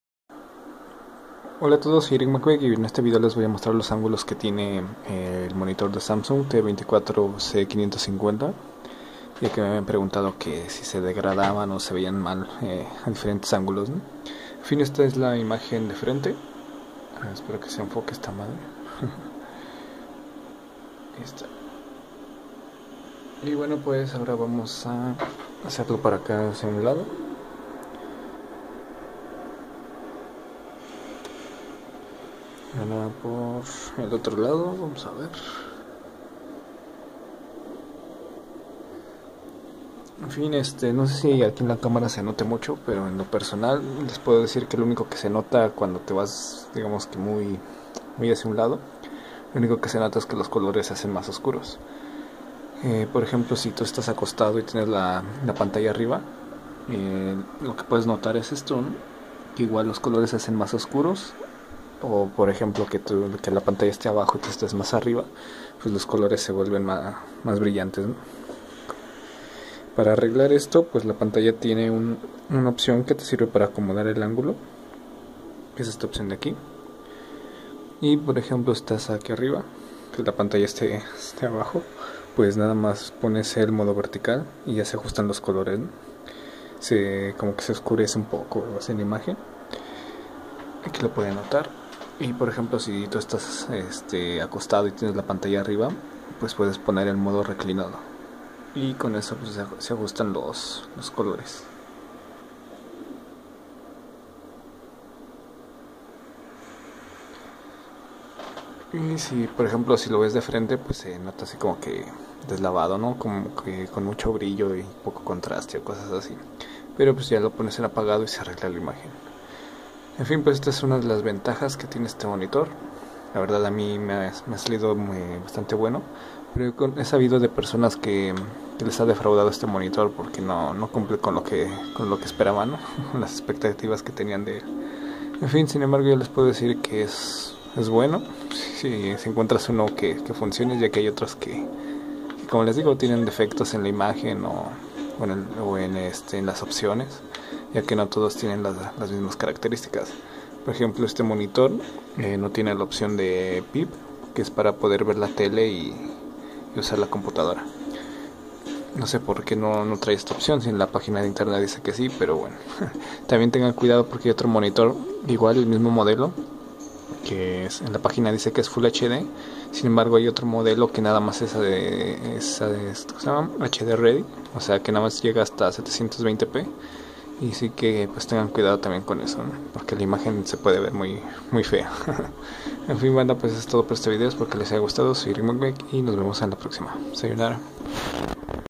Ángulos de visión - samsung tv monitor t24c550
y a  por su membresia!
Meta de suscriptores: . 17% 17.2K/100K

Un vídeo para mostrar los ángulos de visión de este monitor, lo mejor es que lo vean en persona en una tienda y comprueben ustedes mismos la calidad de la pantalla